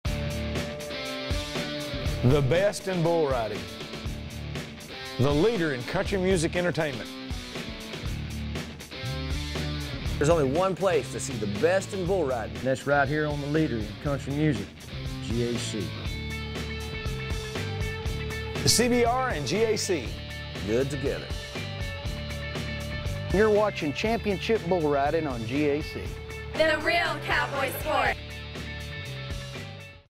CBR Celebrity Promo
Rodney Carrington, Trent Willmon, Craig Morgan, Jason Meadows and our very own...Dallas Cowboy Cheerleaders!